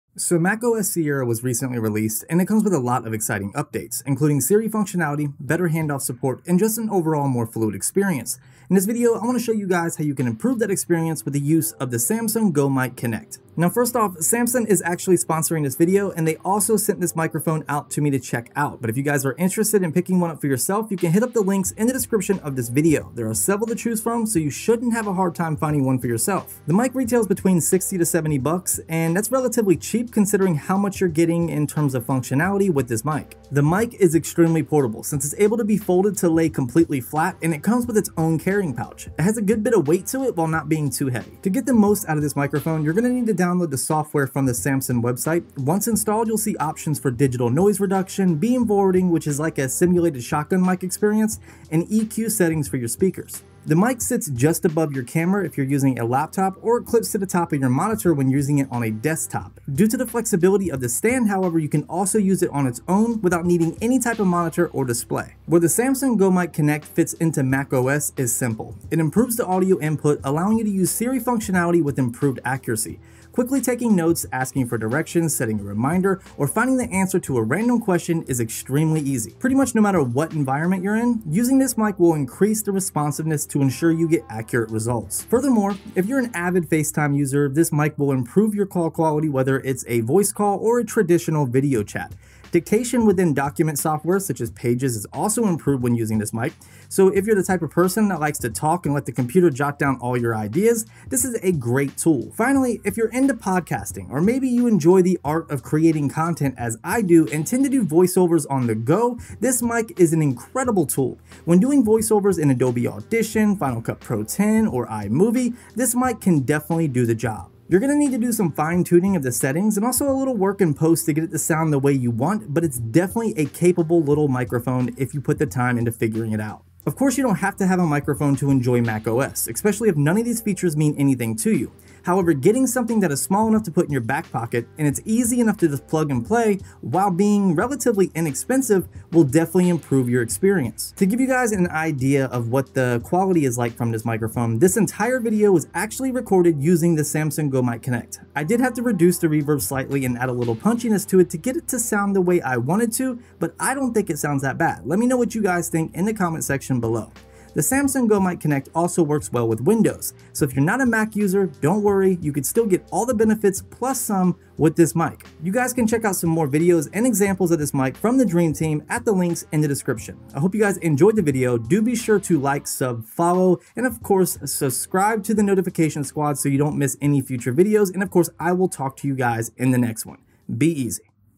Improve Your MacOS Sierra Experience With The Samson Go Mic Connect
MacOS Sierra was recently released and it comes with a lot of exciting updates including Siri functionality, better handoff support, and just an overall more fluid experience. In this video I want to show you guys how you can improve your experience with the use of the Samson Go Mic.